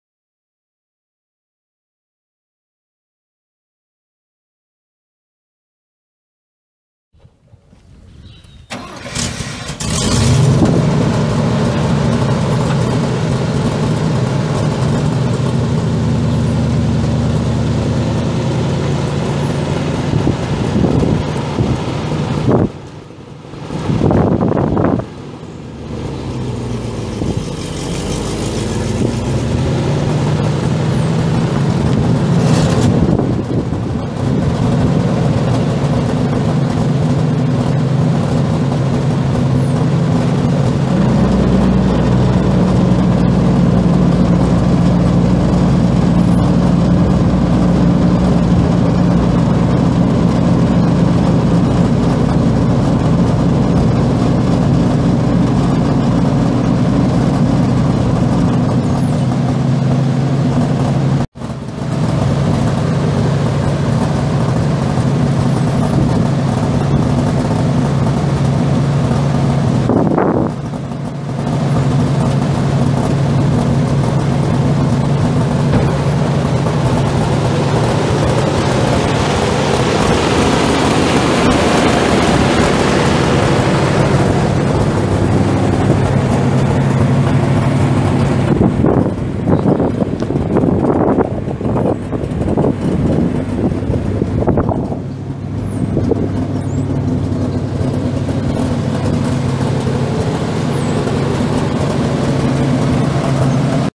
1969 International 1600 grain truck for sale | no-reserve Internet auction May 3, 2017
1969 International 1600 grain truck
55,567 miles on odometer
Eight cylinder gas engine
Manual transmission
Two speed rear end
Heat
AM
Manual windows and locks
Vinyl interior
Chelsea PTO
Knapheide grain bed
16'L x 93"W
40" side height
Wood floor
Steel sides
175" wheelbase
8.25-20 tires
GVWR 19,700 lbs
Kansas antique title
416060H793995
It's located in Sterling, KS, and we're selling it to the highest bidder regardless of price.